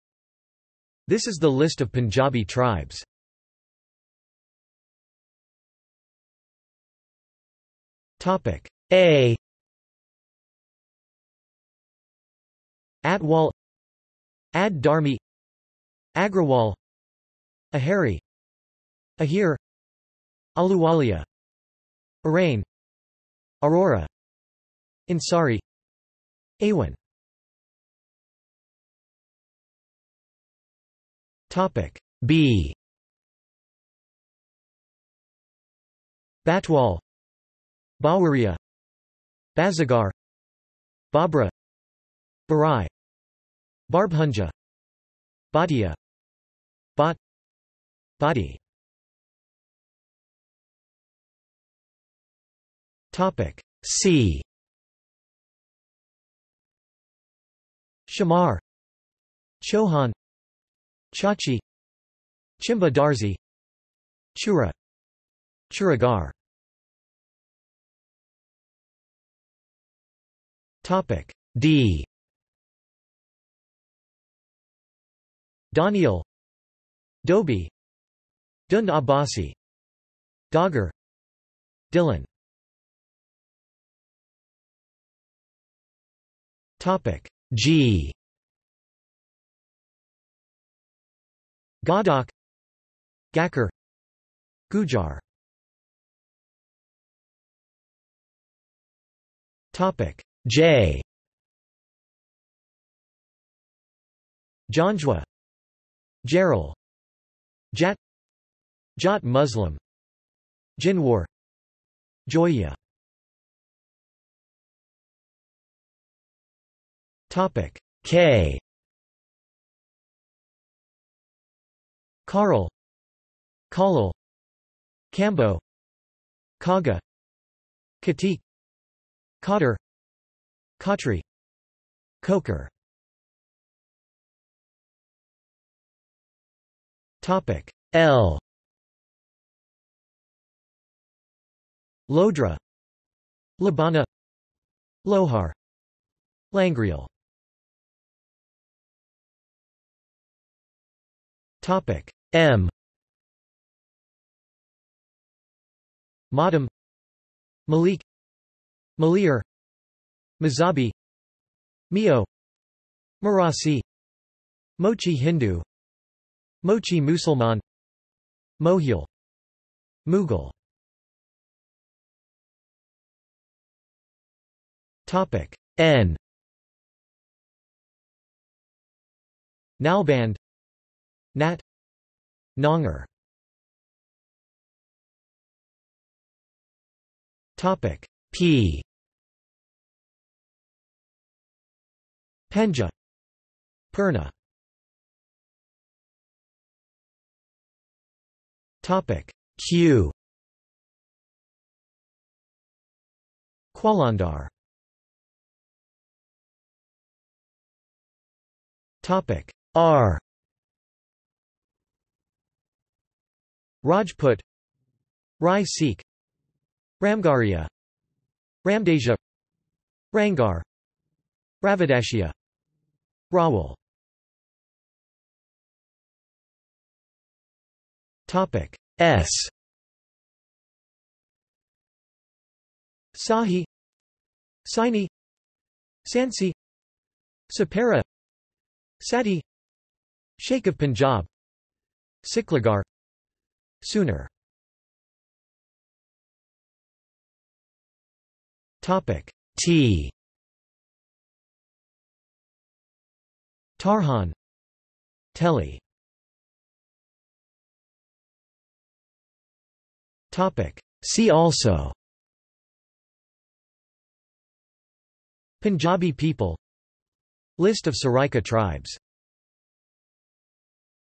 Punjabi tribes | Wikipedia audio article
This is an audio version of the Wikipedia Article:
Punjabi tribes

00:00:04 1 A
00:00:27 2 B
00:00:49 3 C
00:01:07 4 D
00:01:23 5 G
00:01:36 6 J
00:01:53 7 K
00:02:13 8 L
00:02:28 9 M
00:02:51 10 N
00:03:04 11 P
00:03:15 12 Q
00:03:25 13 R
00:03:44 14 S
00:04:05 15 T
00:04:16 16 See also

- Socrates

SUMMARY
This is the list of Punjabi tribes.